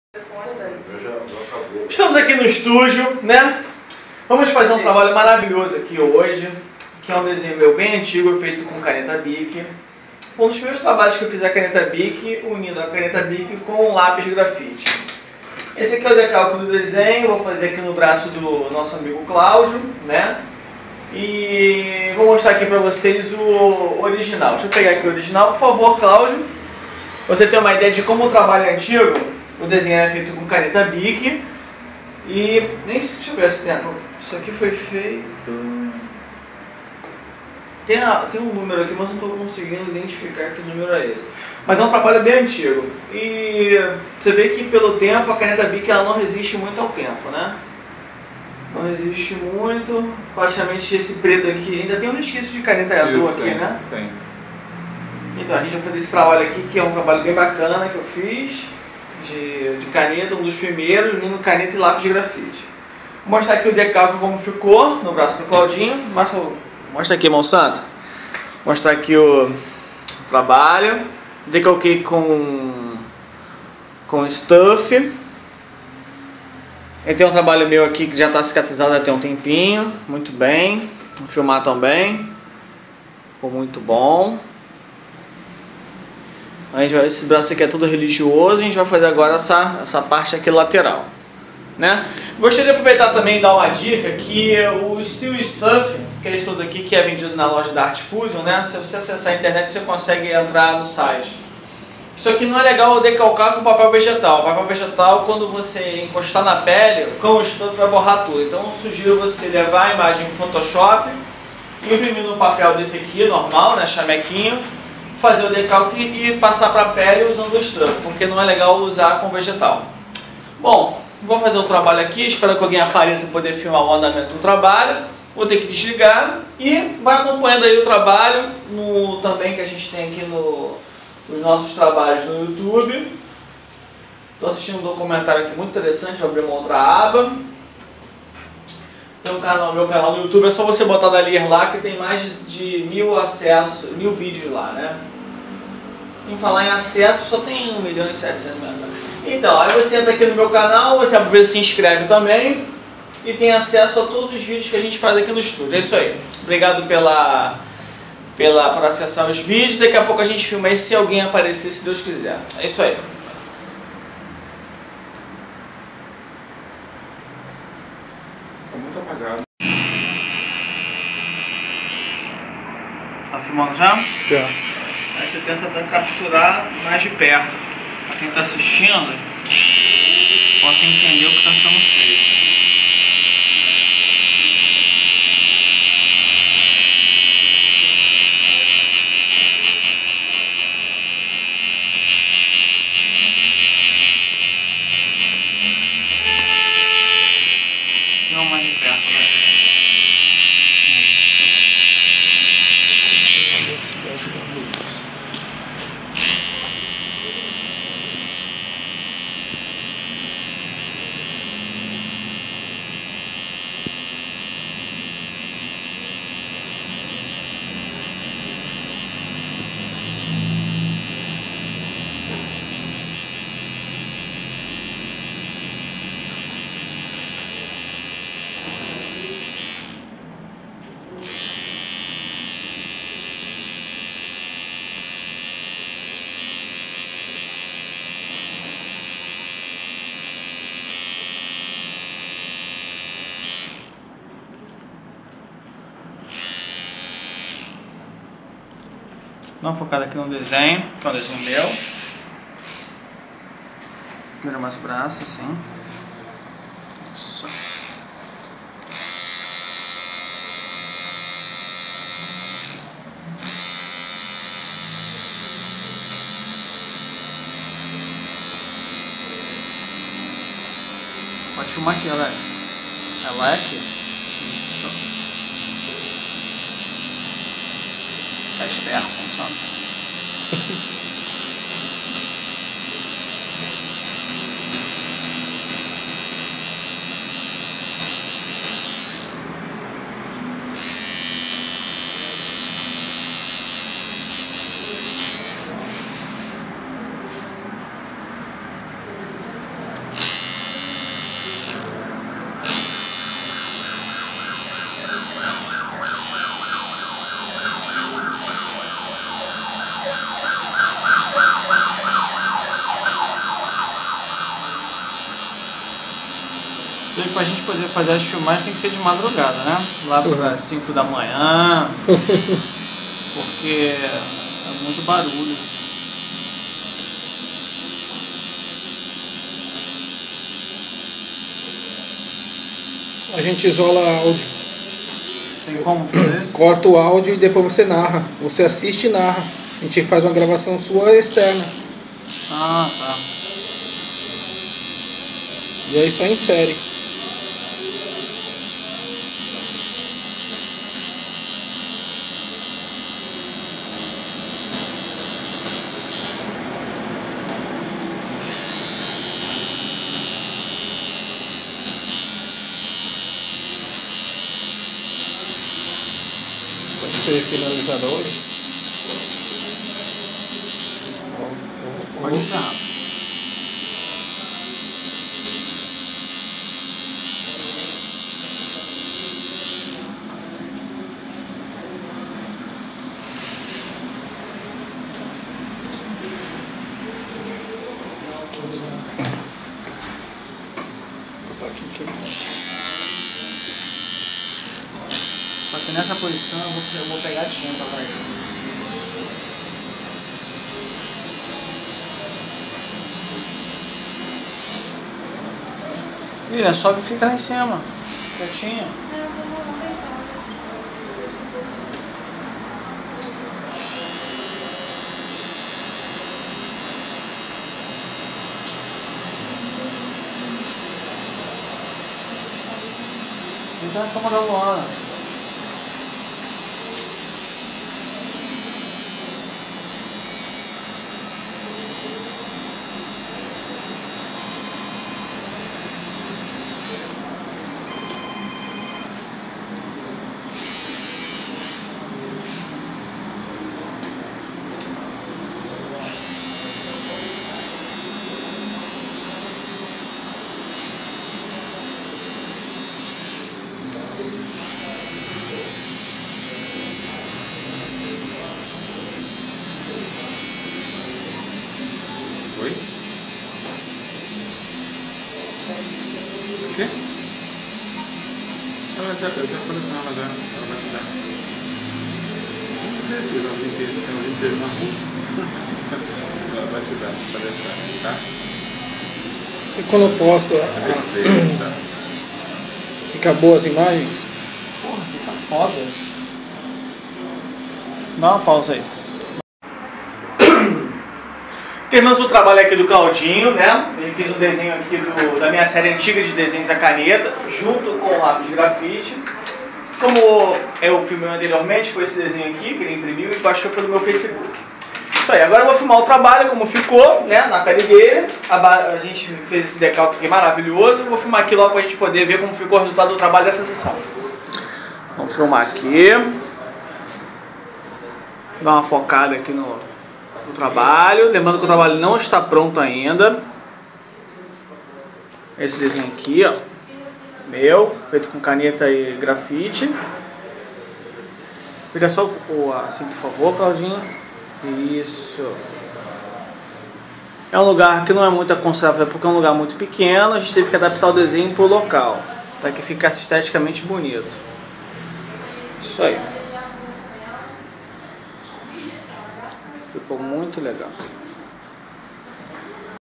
Dallier - Tattoo do Claudio - Editado
Usamos a referencia de um desenho meu antigo feito a caneta que ja ate estava desbotado.
Editamos e espero que gostem. Por Alexandre Dallier